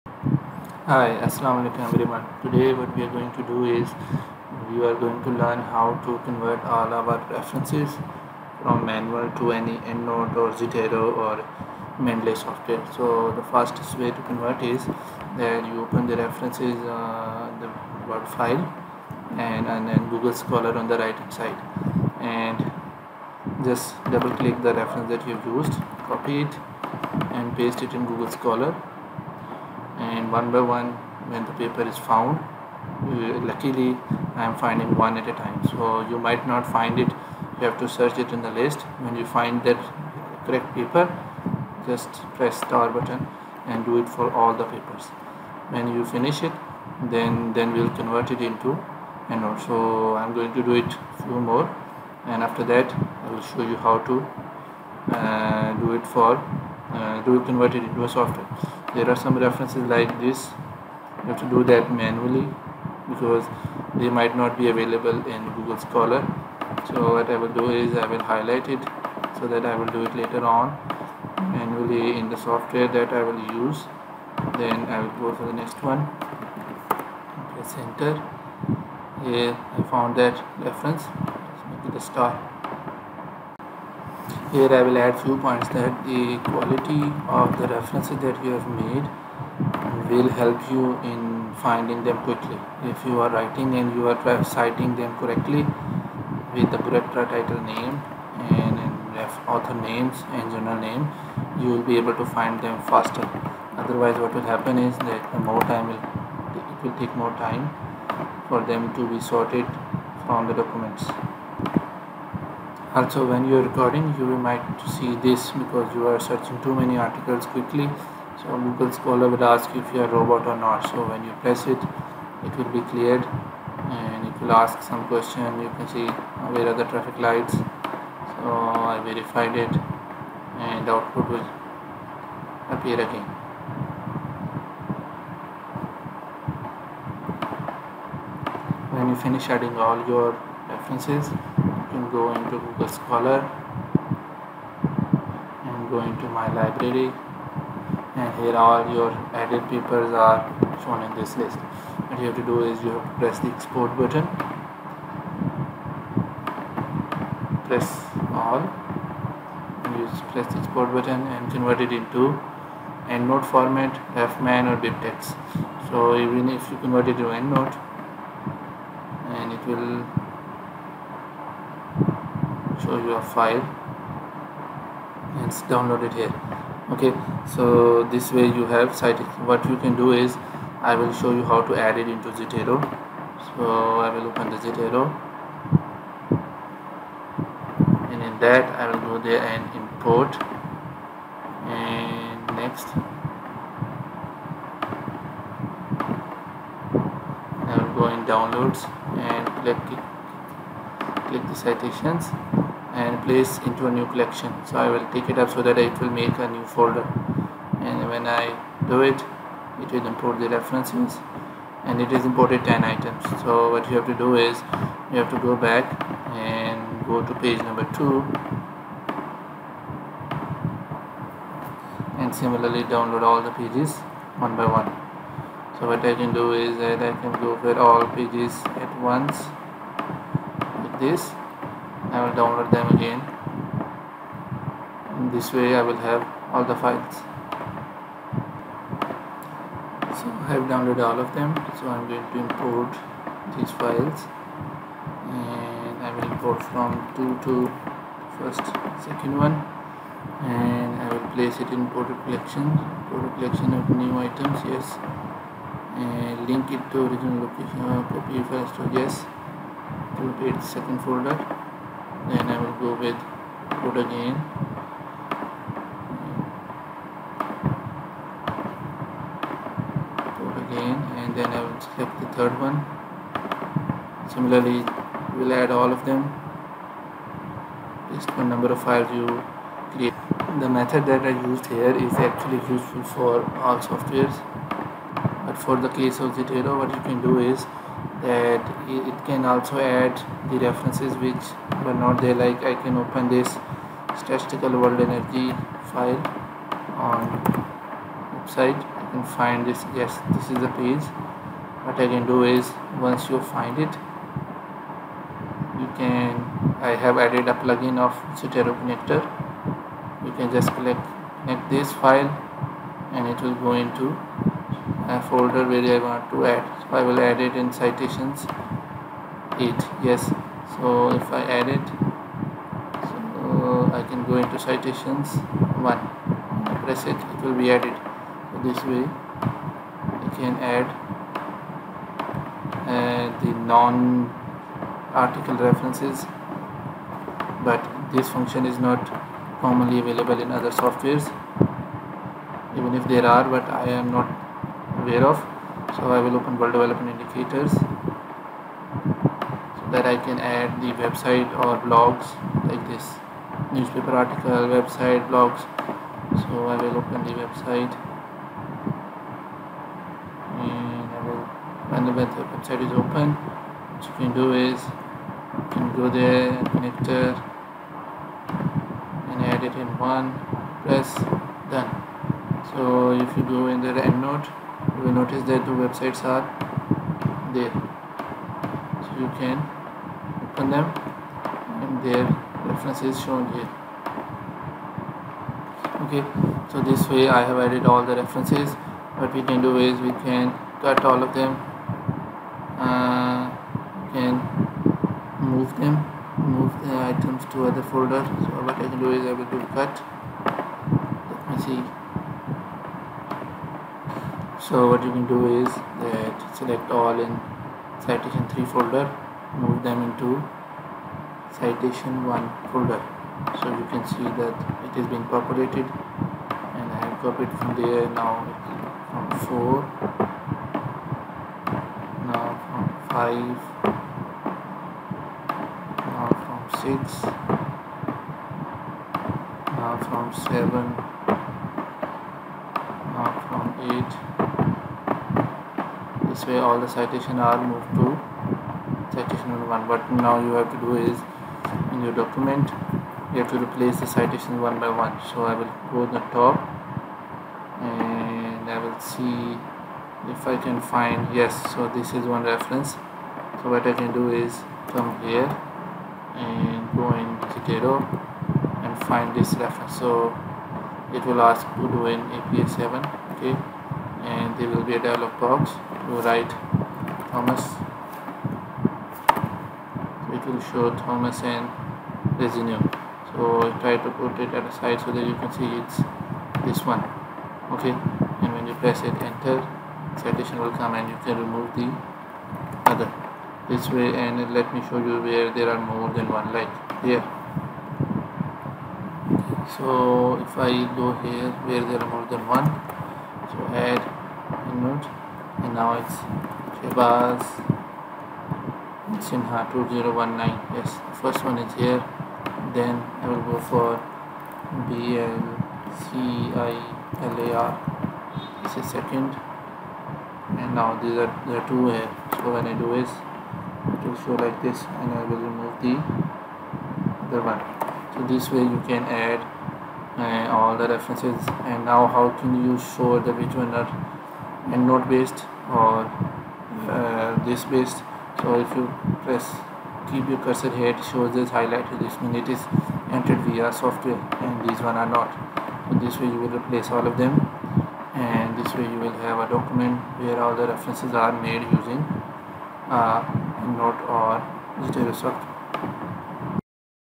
Learn Zotero Citation & Reference Management - Transform Manual References to Referencing Software
Learning  Zotero can make your Citation process Quick! It can Help you to Transform Manual References into Referencing Software...
Using referencing software like Endnote, Mendeley and Zotero are need of the time as they help in managing references and standardizing them. This video helps in importing the references from google scholar.